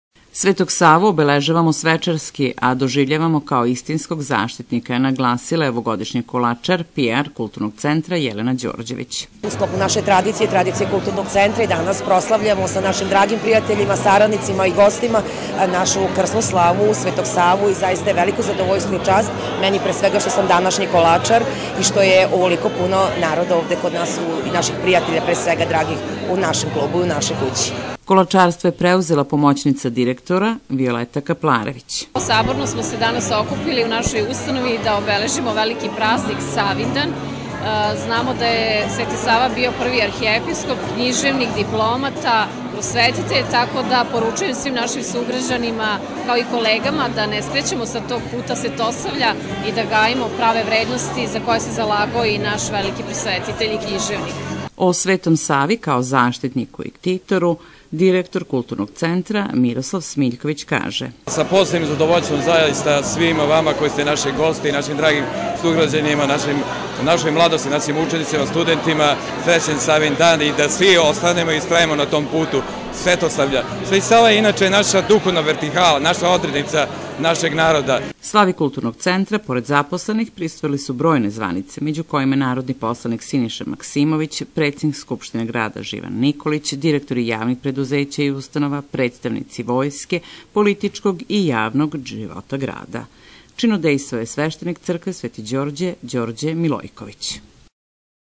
U Kulturnom centru Krusevac proslavljen zastitnik ove ustanove Sveti Sava
"Светог Саву обележавамо свечарски а доживљавамо као истинског заштитника" нагласила је овогодишњи колачар, пр Културног центра Крушевац, Јелена Ђорђевић.

Колачарство је преузела помоћница директора Виолета Капларевић.

Слави Културног центра Крушевац, поред запослених, присуствовале су бројне званице међу којима и народни посланик Синиша Максимовић, председник Скупштине града Живан Николић, директори јавних предузећа и установа, представници Војске, политичког и јавног живота града.

Прилог: Регионална телевизија Крушевац